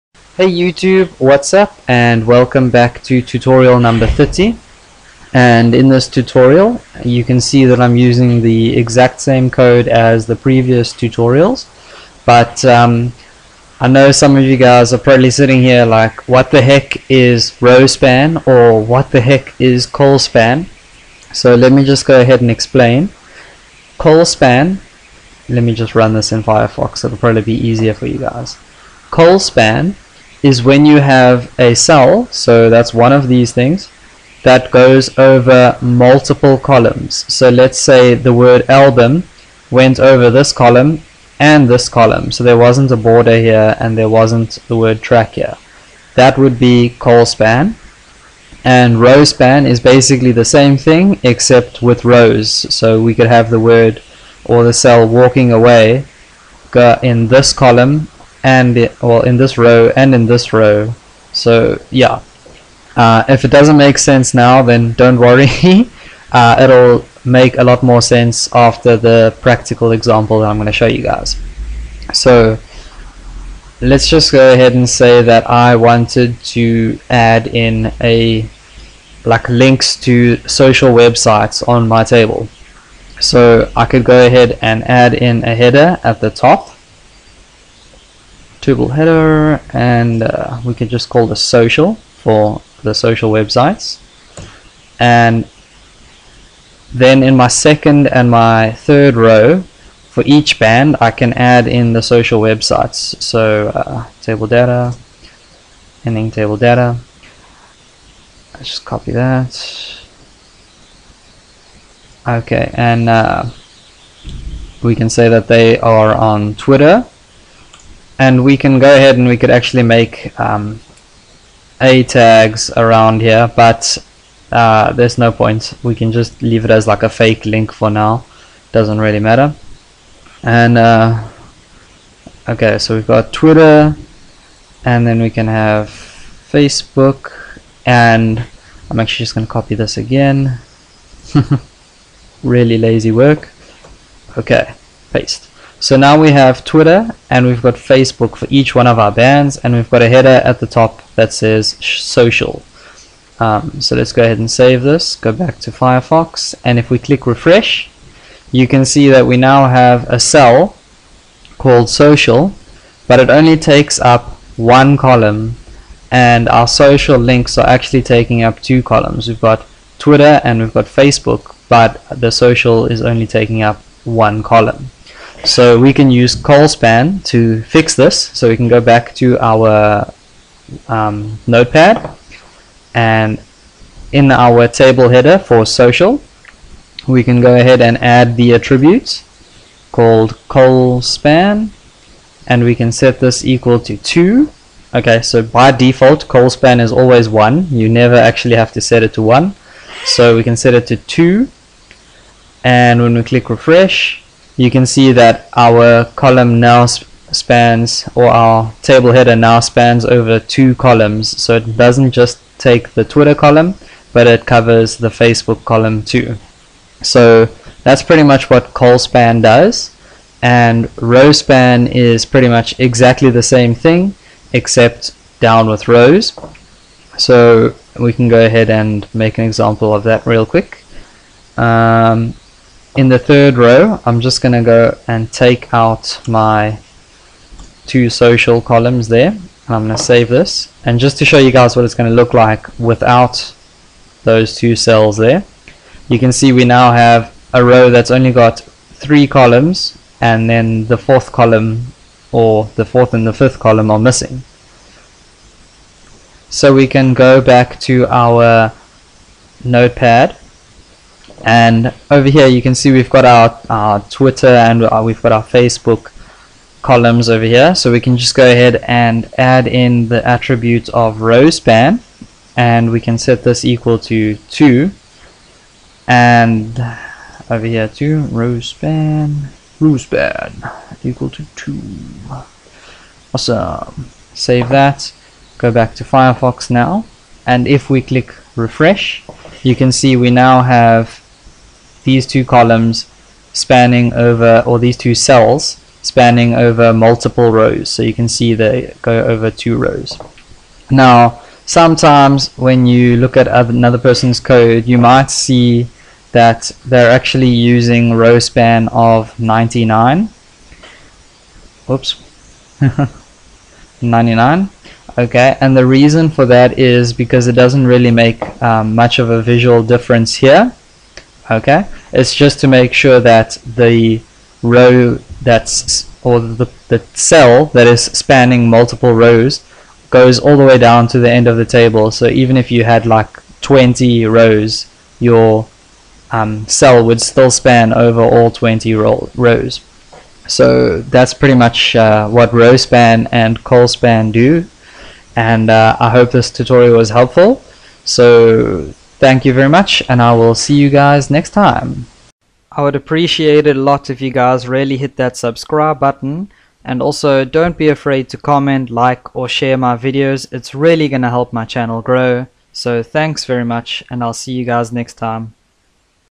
HTML5 and CSS3 Beginner Tutorial 30 - Tables, rowspan and colspan
In this tutorial I show you guys how to make cells span over multiple rows and columns. If you don't know what rowspan and colspan are then, don't worry, I explain that too.